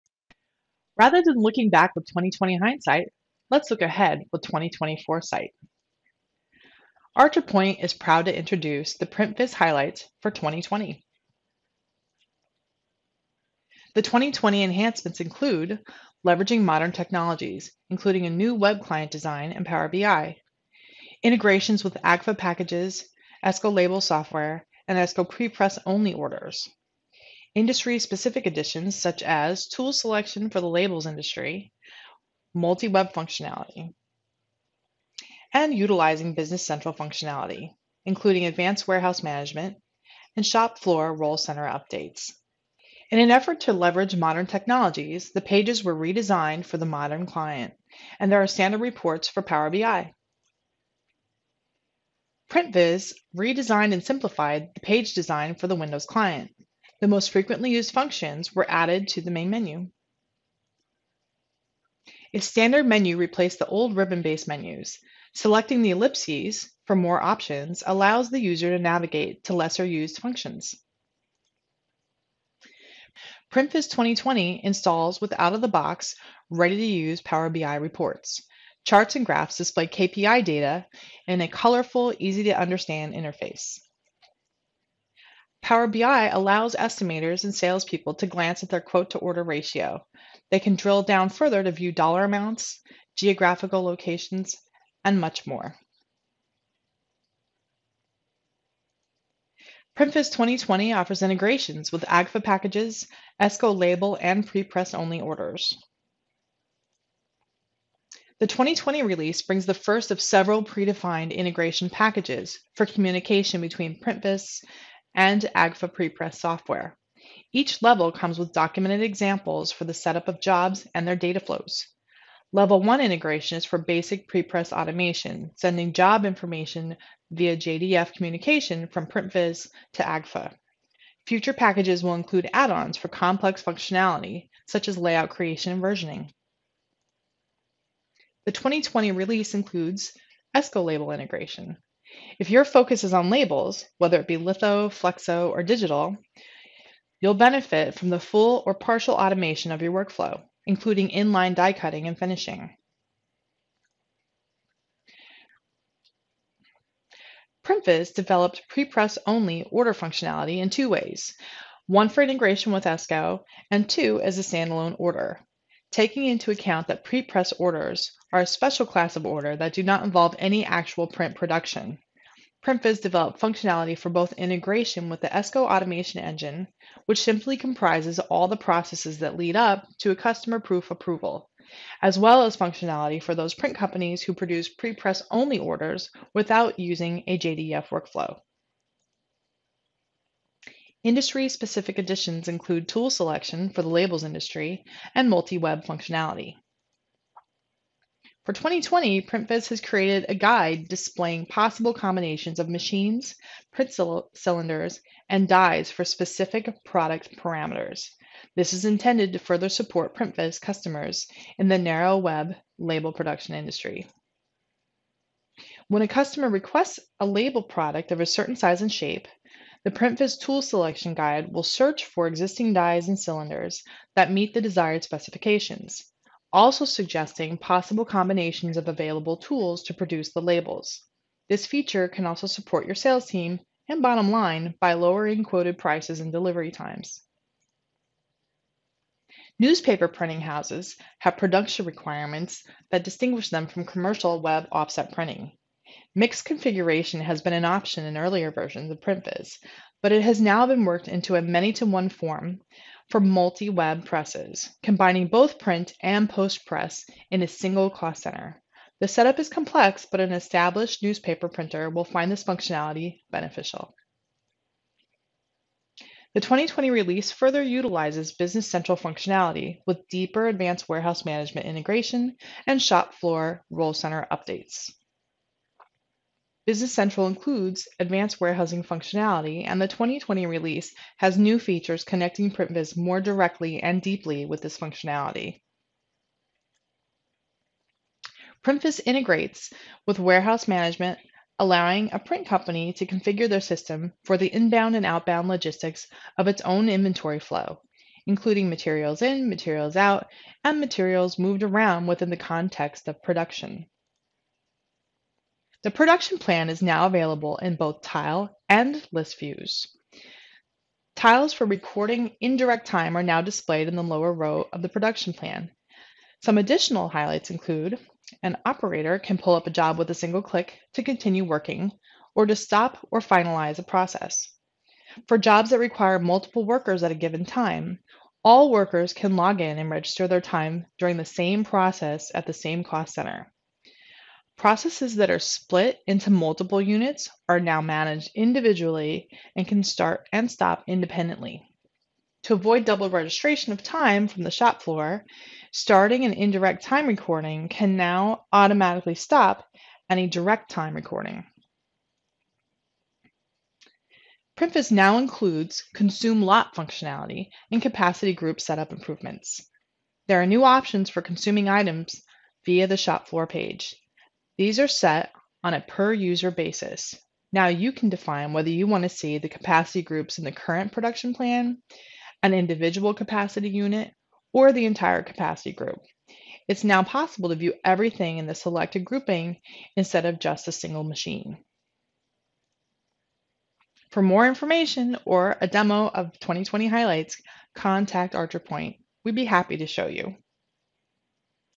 PrintVis ERP for the Printing Industry: 2020 Enhancements
Learn about the highlights and enhancements of PrintVis, a Microsoft-powered ERP, specifically designed for the printing industry. PrintVis enhancements include:
- Modern user interface for easier, more streamlined navigation
- Integration with external program (like AGFA, Esko, prepress)
- Leverages Power BI with more standard reports that are easy to use and understand
- Shop floor
- WMS
See what's new and improved in PrintVis 2020